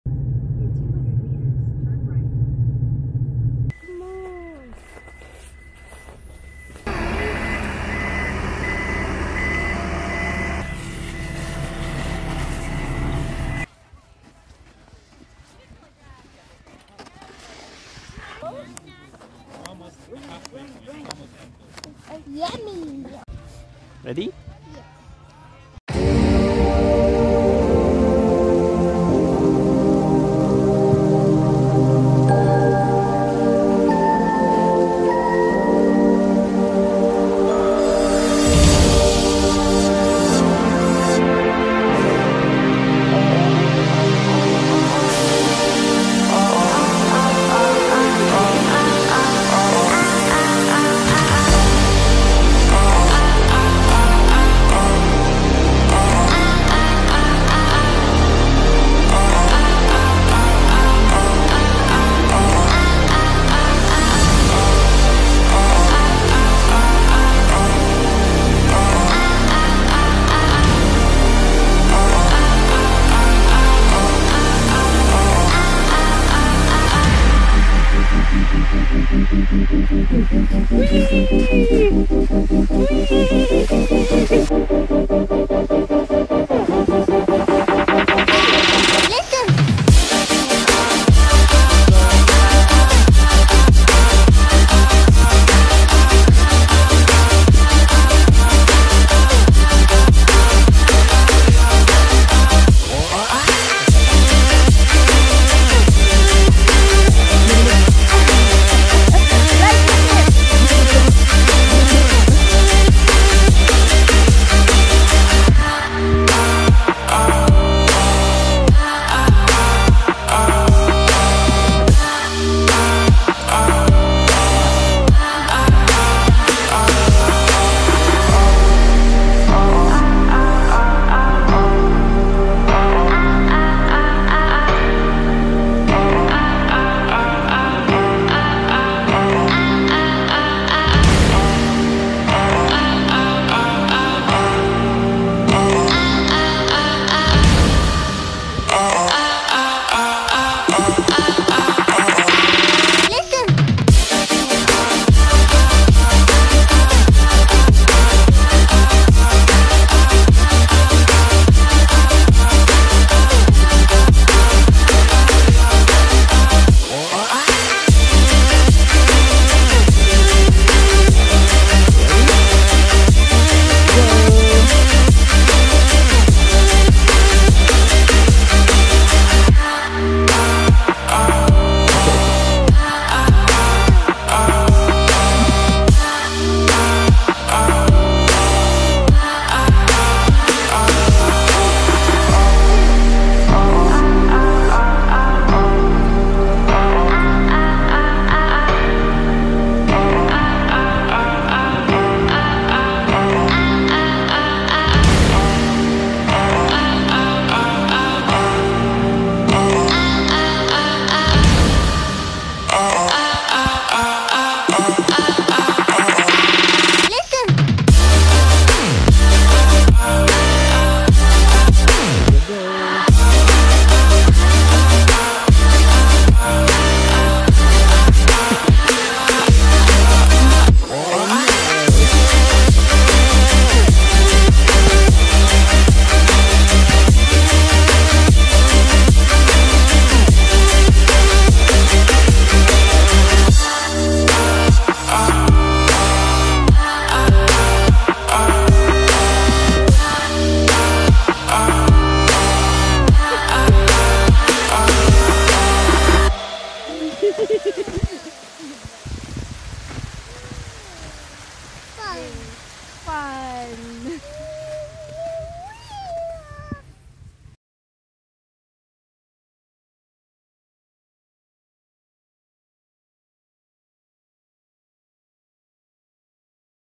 Mt Seymour Snowboarding
Tate's 1st try snowboarding @ 2years old. He really loves it!